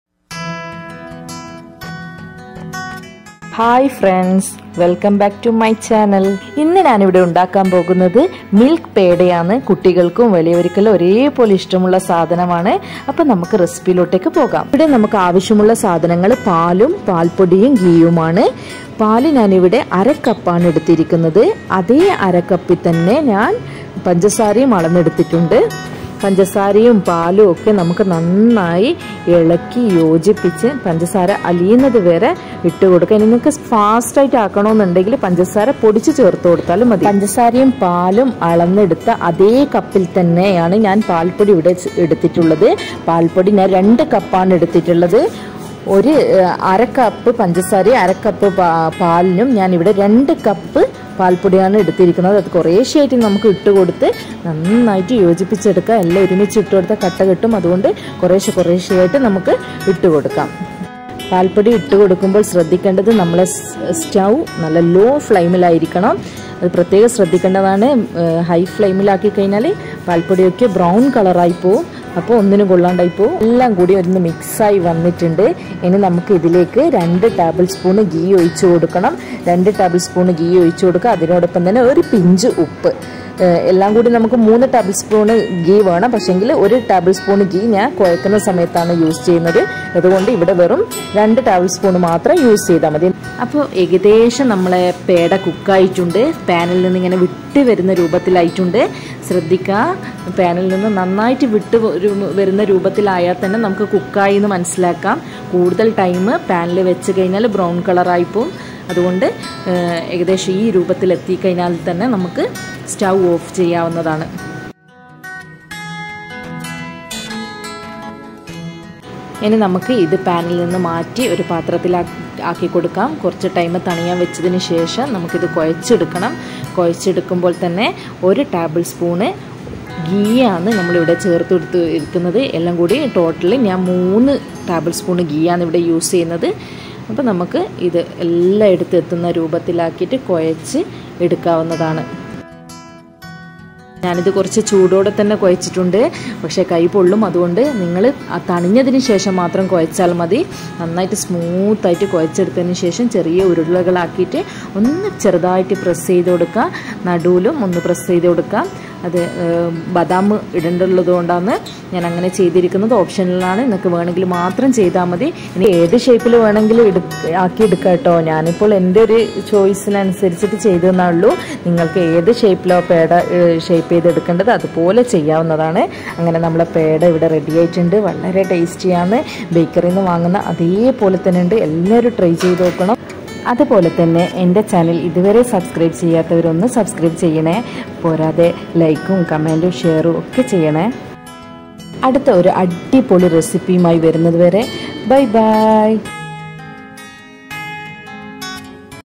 വെറും 3 ചേരുവകൾ കൊണ്ട് മിൽക്ക് പേട തയ്യാറാക്കാം || MILK PEDA
ingredients 👇🏻👇🏻
milk powder : 2cup
milk :1/2 cup
 sugar: 1/2 cup
ghee : 3tbsp
SALT :pinch